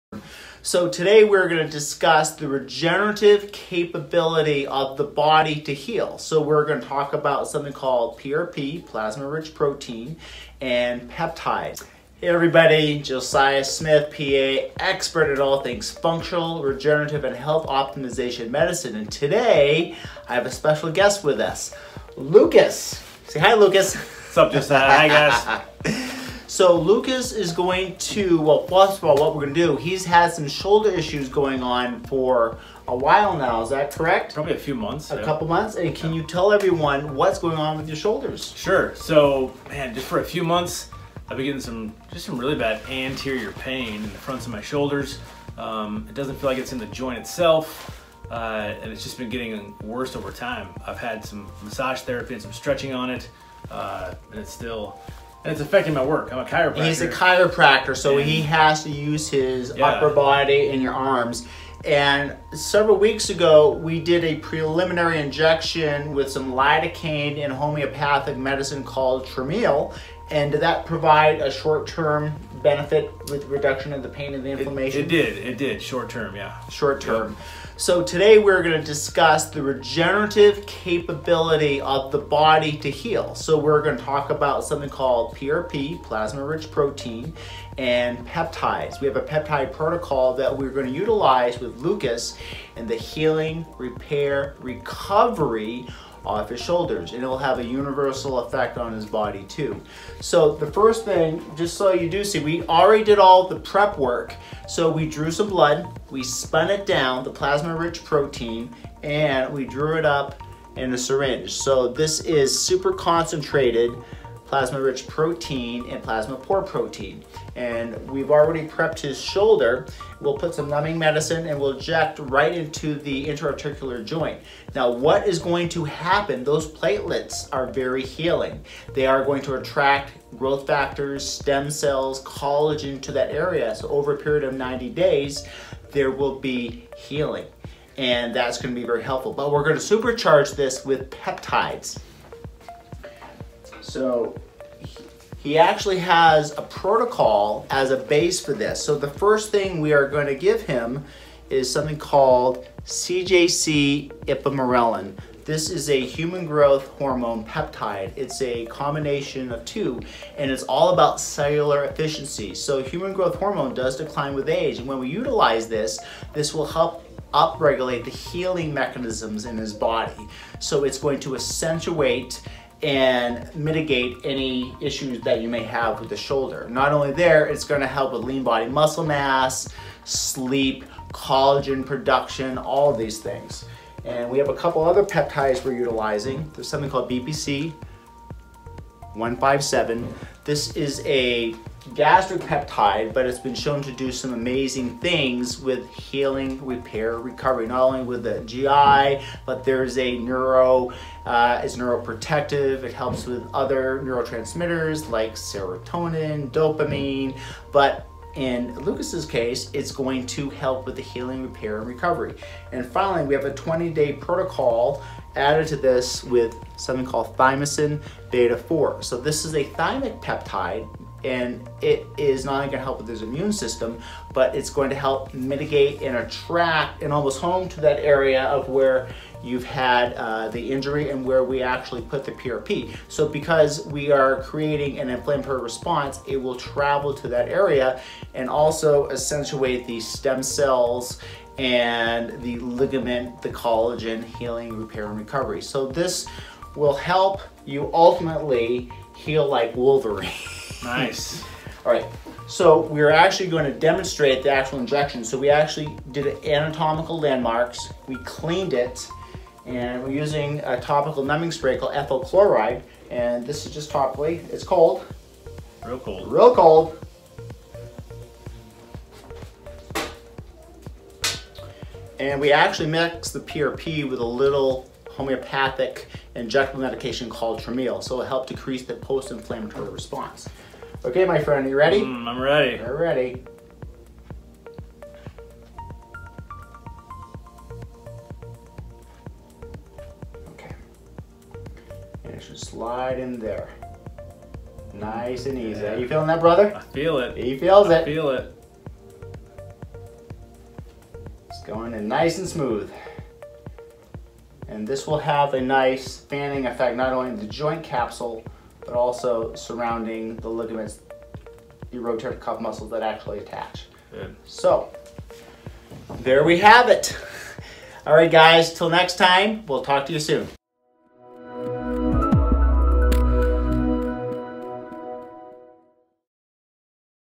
PRP & HEALING PEPTIDES FOR SHOULDER PAIN
Learn how Lucus is using his own blood / Plasma Rich Protein (PRP) and PEPTIDES: CJC-1295/Ipamorelin, BPC-157, and Thymosin B-4 to enhance healing and repair of his shoulder tendonitis.